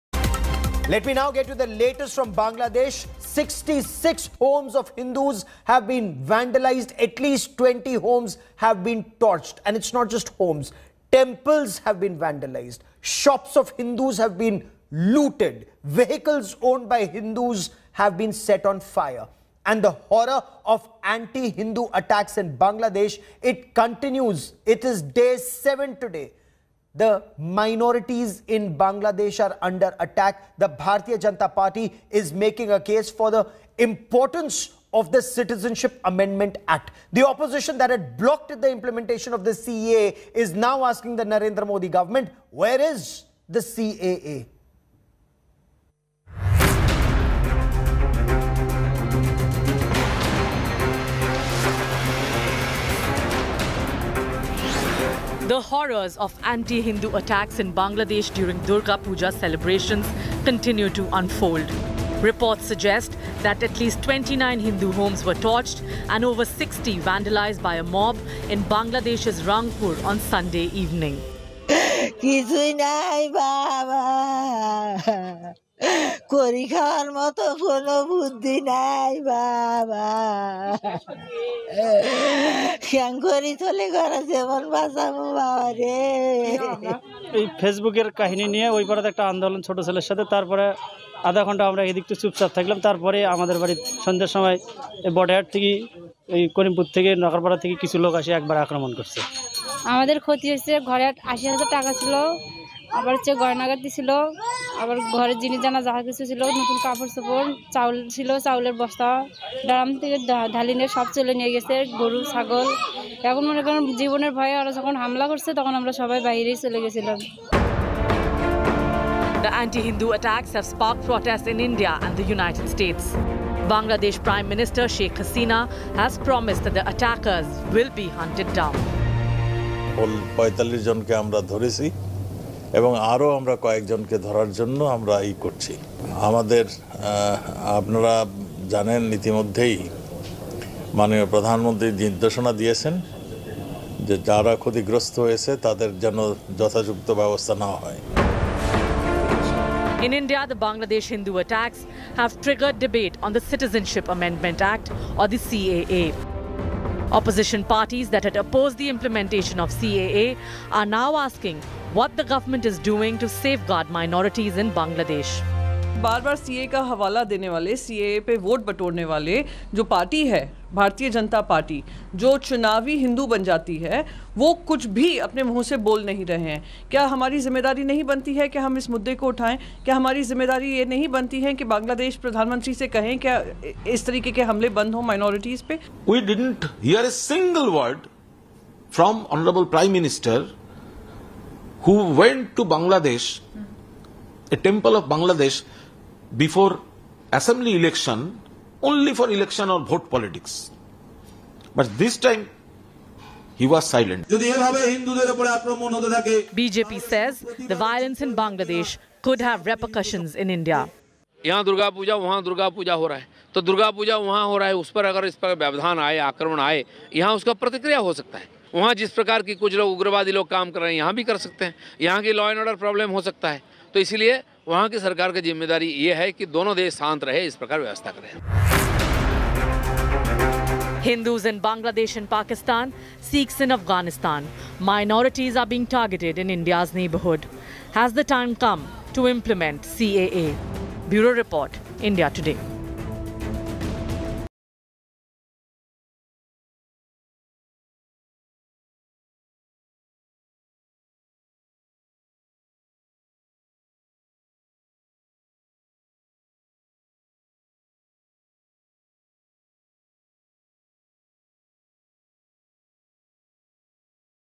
Bangladesh Hindu Attacks: Will India Open Doors To Persecuted Hindus? | India Today
A group of assailants vandalised at least 66 homes and torched 20 homes of Hindus in Bangladesh over an alleged blasphemous social media post. This comes amid outrage over incidents of temple vandalism in the country.